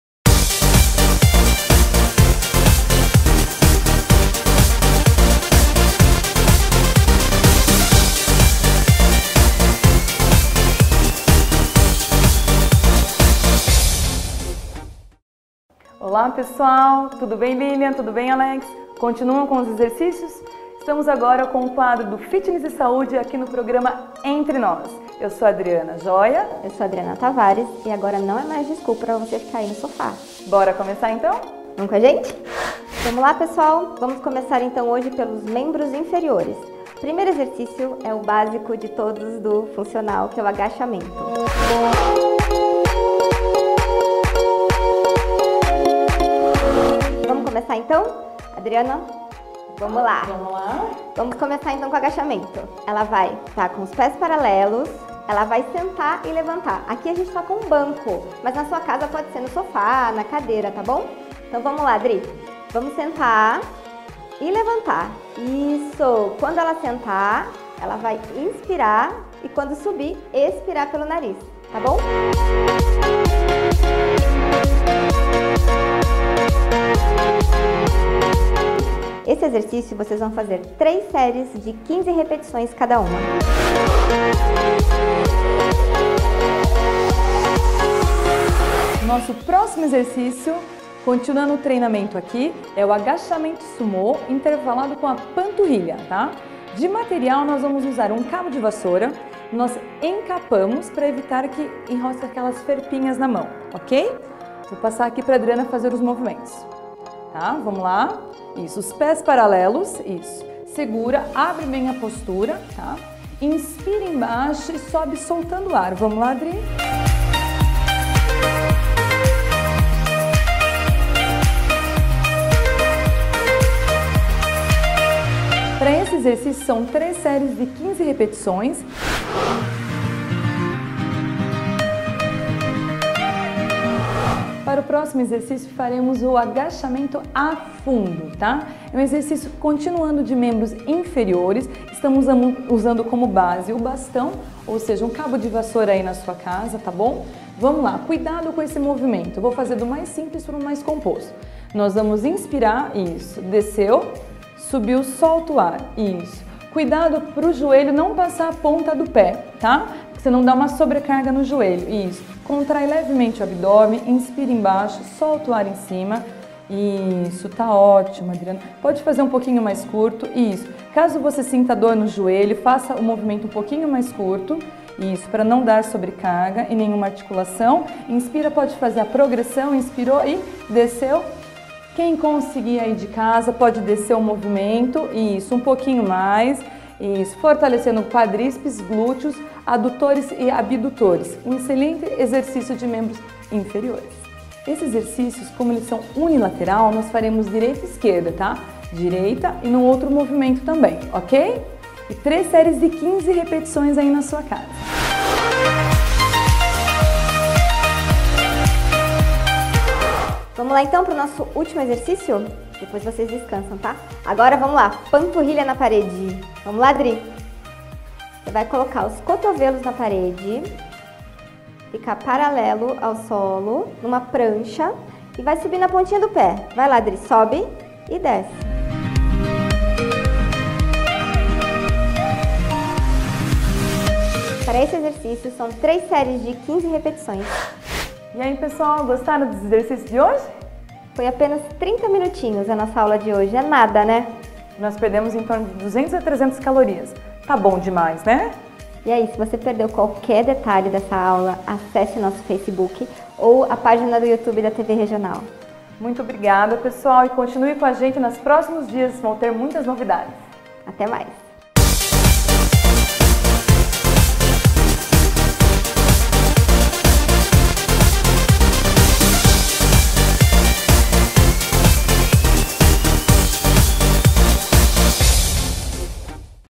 Fitness e Saúde Personal - Aula 1, Membros inferiores - TVR
Aula 1. Membros inferiores

Primeiro exercício:
Agachamento no Banco

Segundo exercício:
Agachamento sumo mais panturrilha com bastão

Terceiro exercício:
Agachamento afundo Curto
Agachamento afundo Completo

Quarto Exercício:
Panturrilha na parede